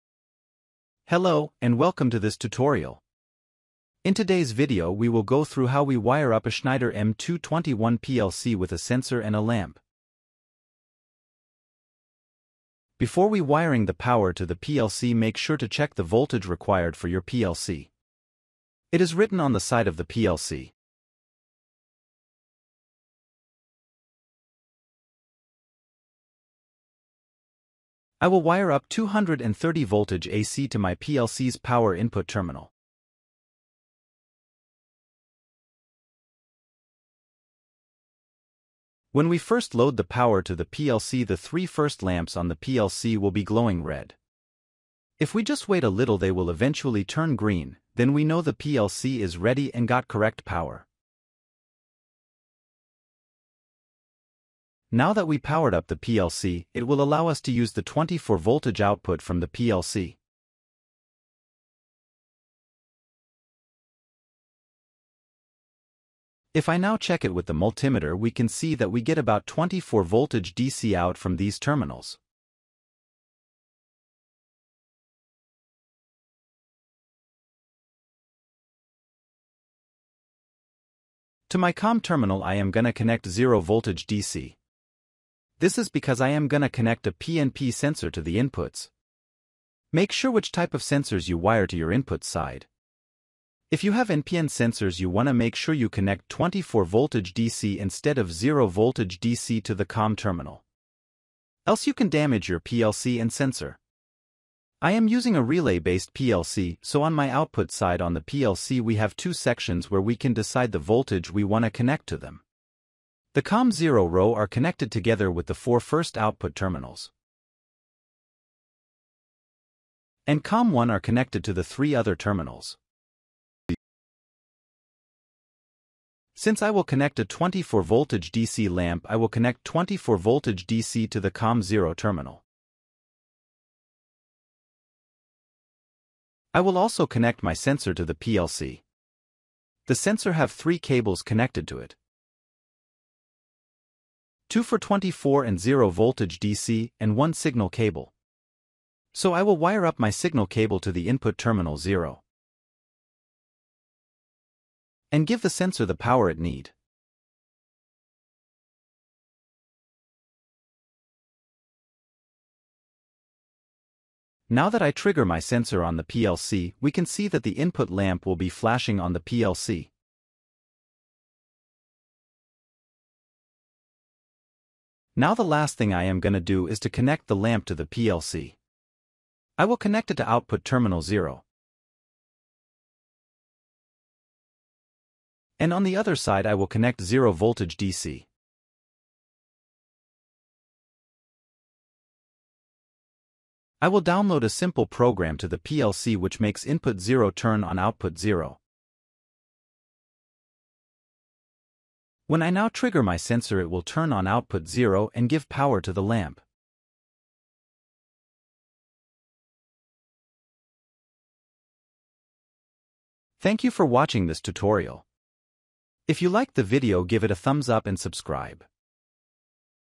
Schneider M221 hardware and wiring tutorial
Whatsup everyone. In this video we will go through how to wire up a schneider m221 PLC with a sensor and a lamp. Hope you learn something new!